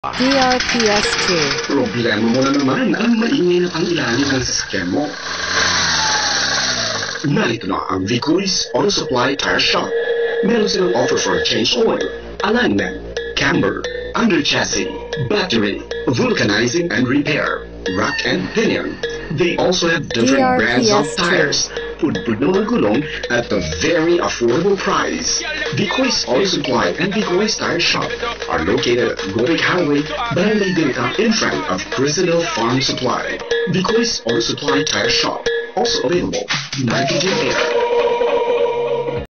Vicoy's Oil and Tire Shop Radio Ad Q4 2024 39s (Iba, Zambales)
Narito po ang Q4 2024 Provincial Radio Ad ng Vicoy's Oil and Tire Shop sa Iba, Zambales.

DRPSTwo 8938: Vicoy's Oil and Tire Shop Radio Ad Q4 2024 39s (Iba, Zambales)